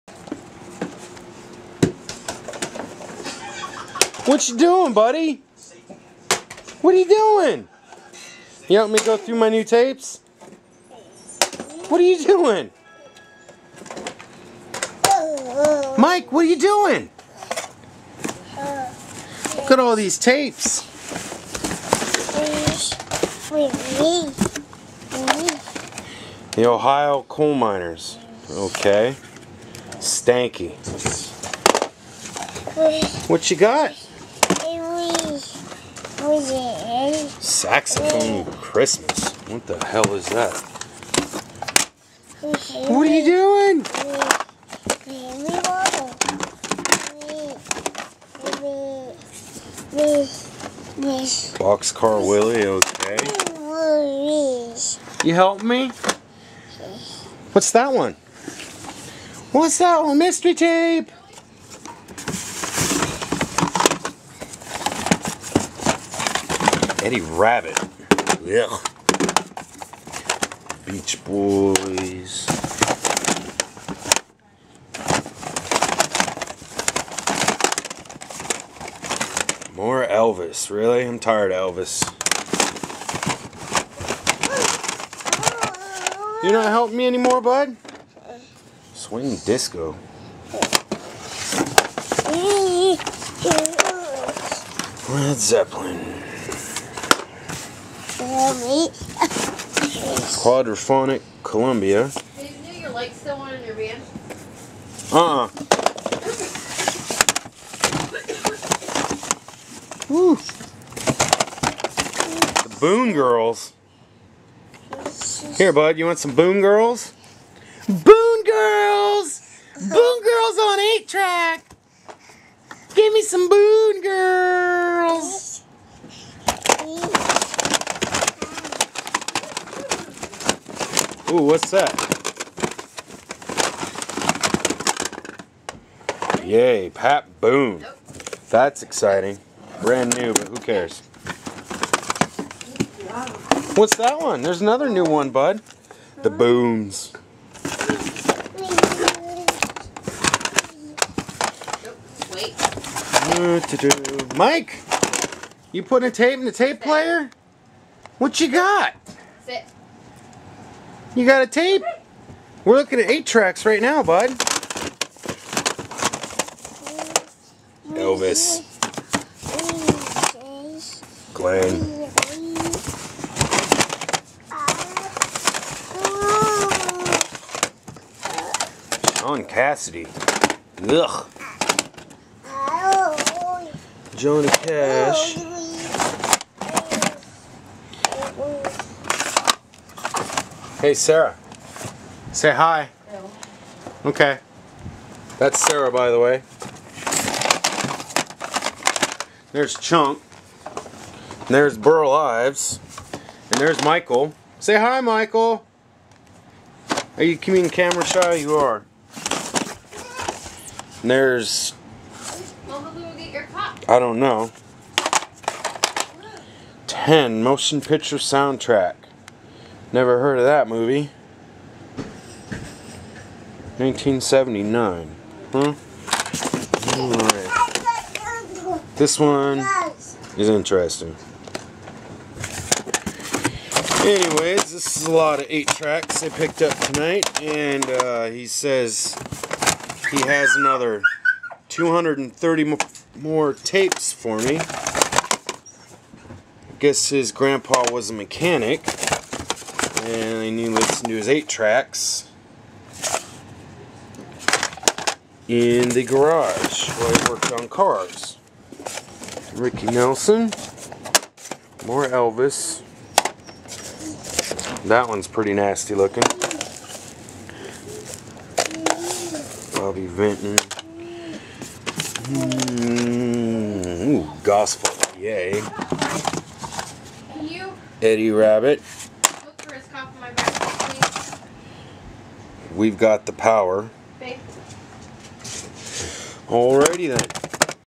Today's 8 Track tape lot 3-1-2013
Just a quick video of some 8 track's i picked up today.